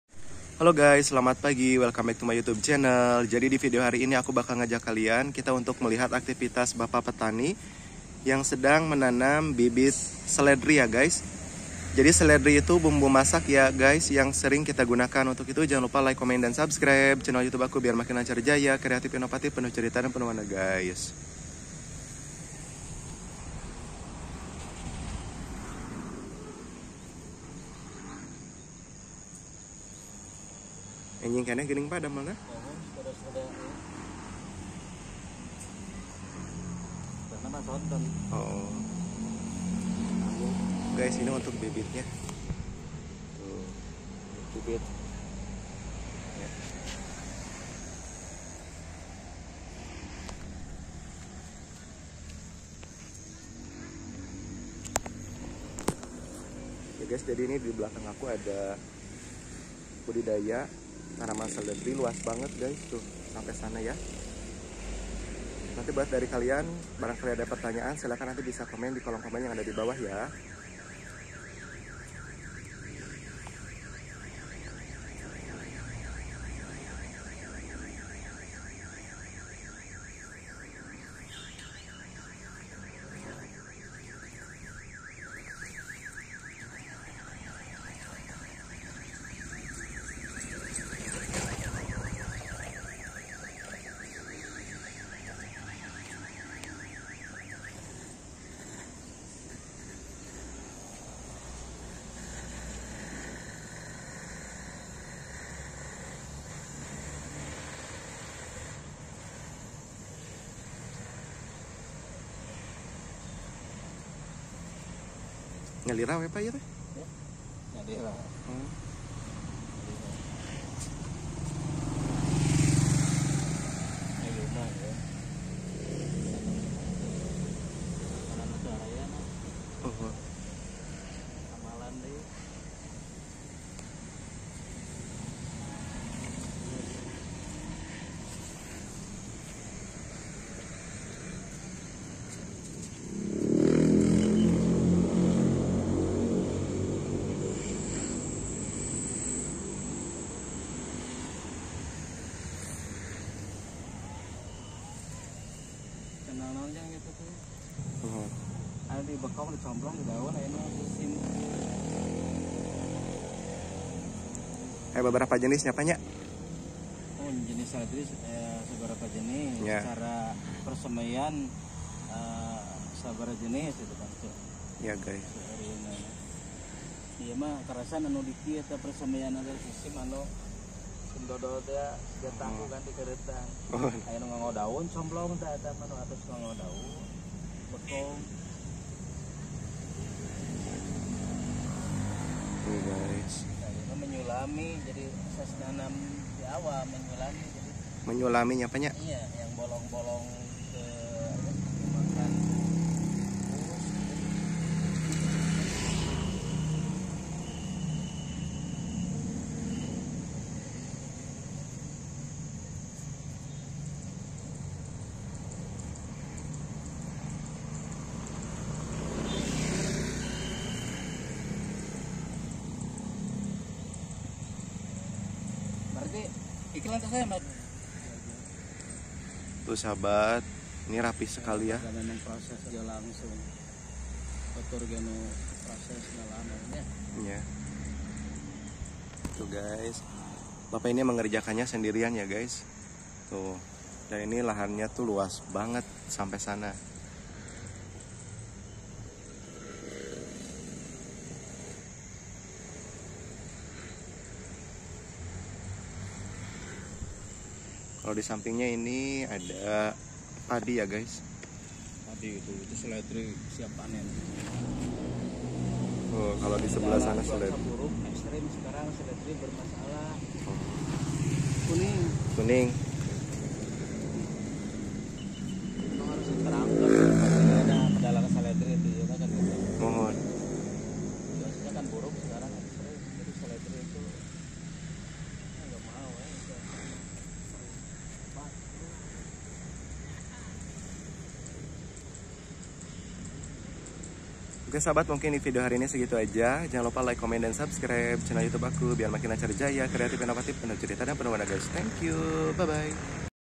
BUDIDAYA DAUN SELEDRI
Oke Sahabat Jadi di video kali ini aku bakal ngajak kalian kembali untuk melihat aktivitas petani kembali di pesawahan di video hari ini Kebetulan petani mau menanam bibit seledri untuk itu ikutin aku terus jangan lupa like comment dan subscribe thank you